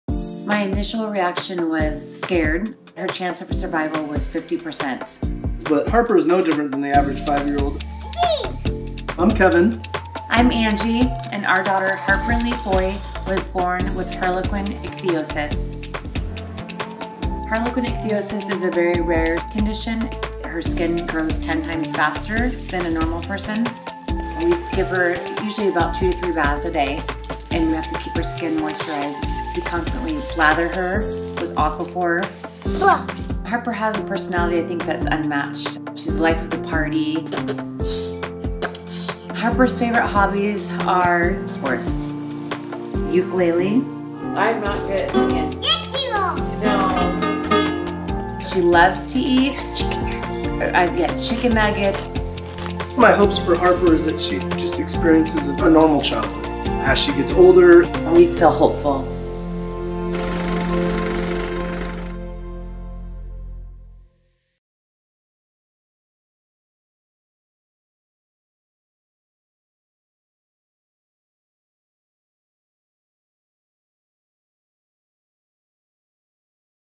Harlequin Ichthyosis | Harper Ly Foy
Harper Ly Foy, now 5, was diagnosed with Harlequin Ichthyosis hours after her birth. The rare disorder covers her in thick skin that restricts breathing and blood flow, so Harper’s parents Angie and Kevin spend four hours a day bathing her and lathering her in soothing balm to make her comfortable. With Harper now signed to a modeling agency, her parents hope that her social media presence will raise awareness of the condition.

60 Second Docs